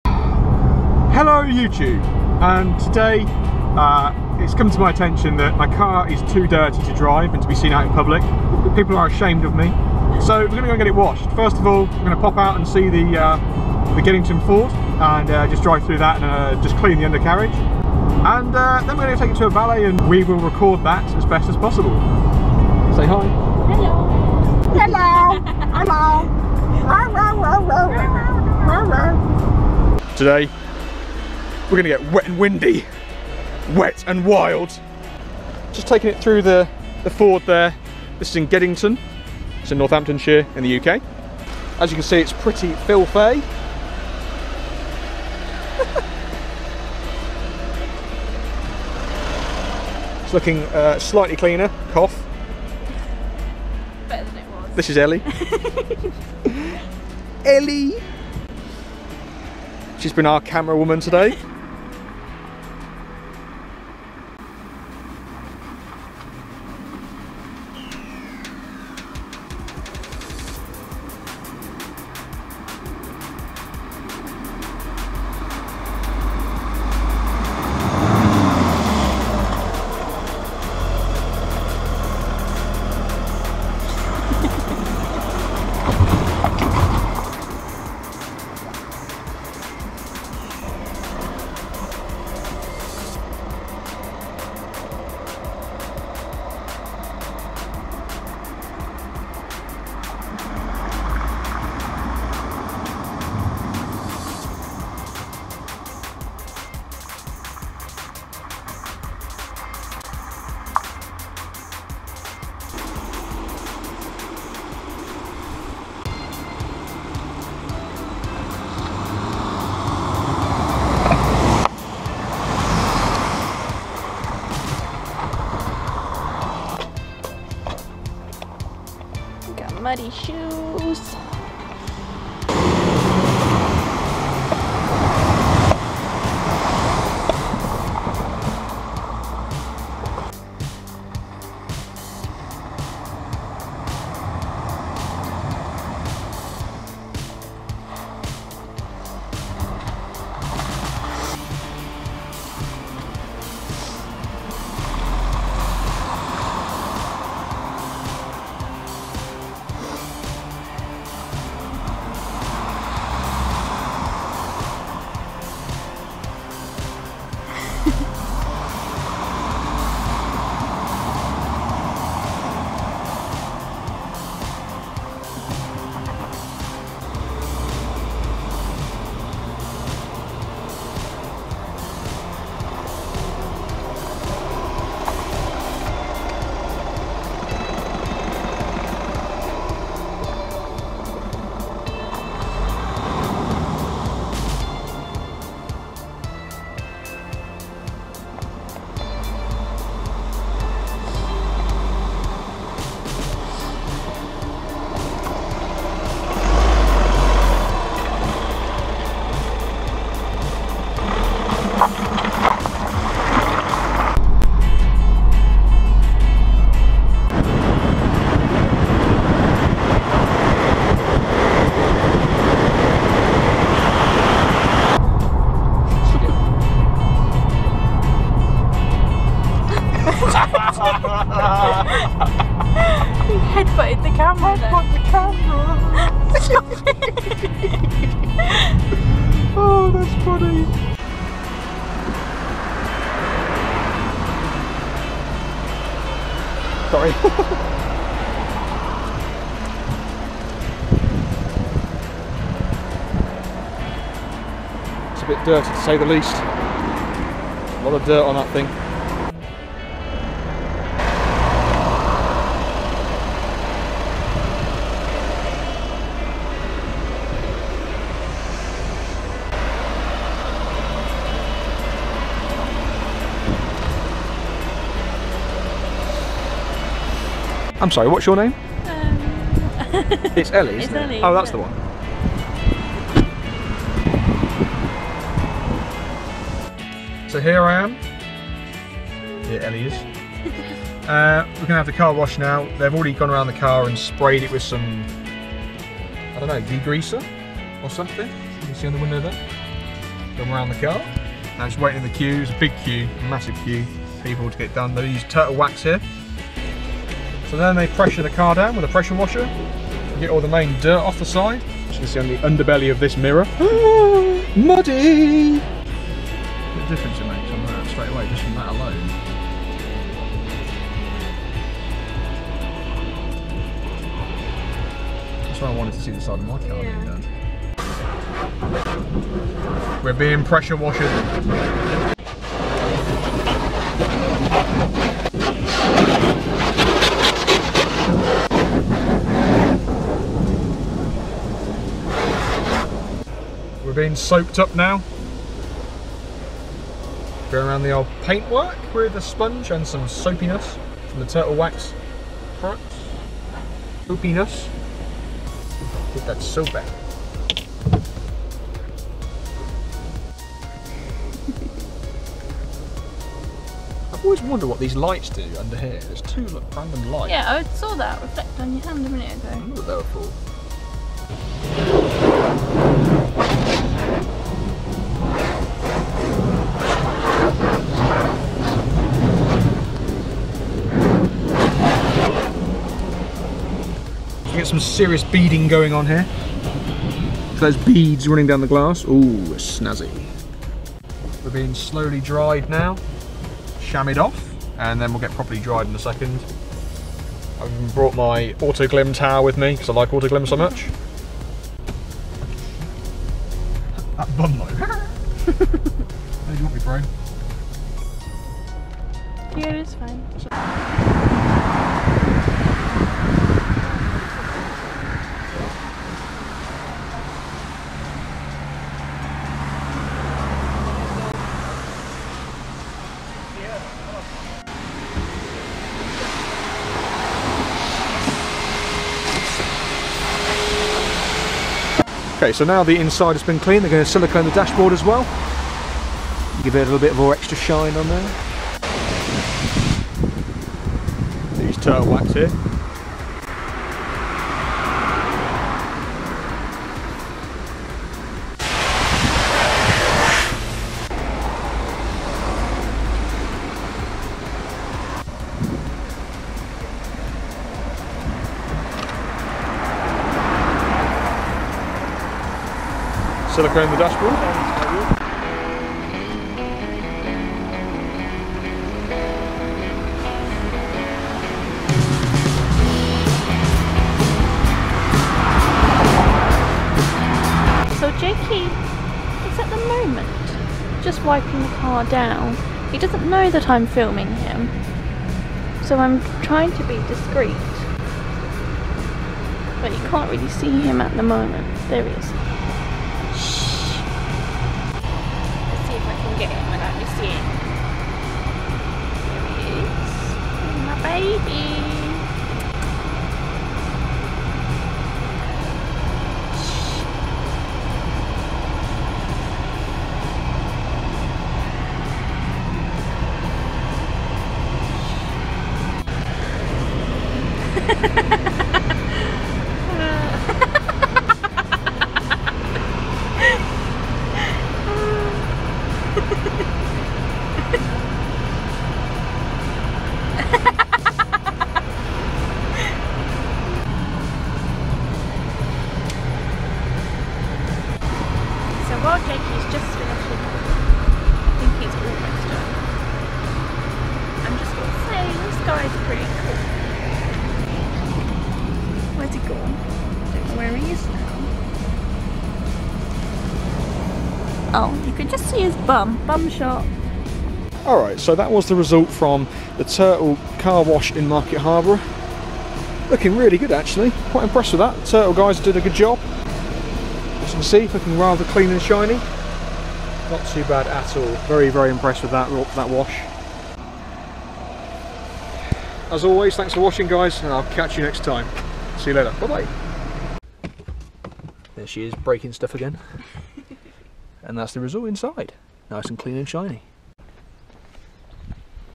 Turtle Hand Car Wash Market Harborough Cleaned My Car
Sunny Sunday afternoons are good for one thing, getting the car washed and hoovered! First of all Lets get it wet in a drive-able river (ford) in Geddington, then take it to a drive through wash and wax center to have it valeted in Market Harborough.
This is a Turtle Wax Hand Car Wash center.
I was happy to give these guys a tip as the car looked great!
Enjoy watching this and remember to give these guys a go if you're in Market Harborough.